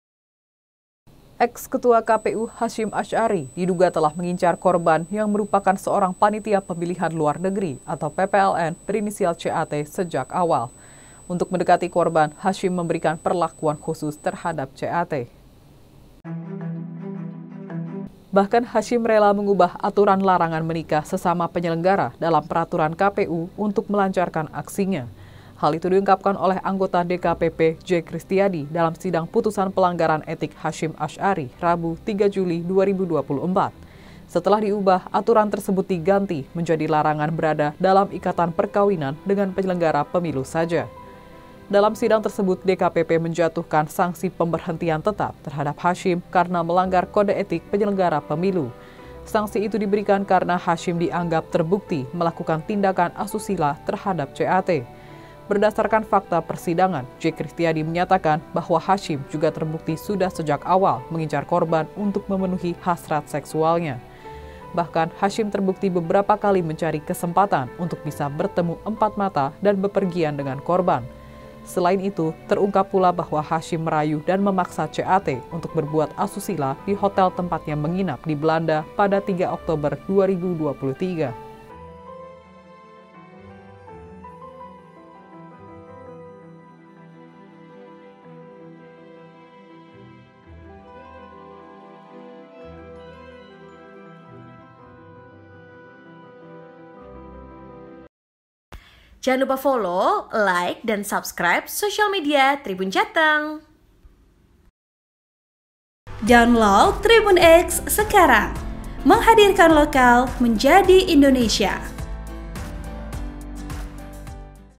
Hasyim Eks Ketua KPU Ubah Aturan Larangan Menikah Sesama Anggota Dekati Korban
Eks Ketua KPU, Hasyim Asy'ari diduga telah mengincar korban yang merupakan seorang panitia pemilihan luar negeri (PPLN), berinisial CAT sejak awal.

Untuk mendekati korban, Hasyim memberikan perlakuan khusus terhadap CAT.

Bahkan Hasyim rela mengubah aturan larangan menikah sesama penyelenggara dalam Peraturan KPU (PKPU) untuk melancarkan aksinya.

Hal itu diungkapkan oleh Anggota DKPP J Kristiadi dalam sidang putusan pelanggaran etik Hasyim Asy'ari, Rabu (3/7/2024).

Host : Elsa Elningtyas
Editor : Yga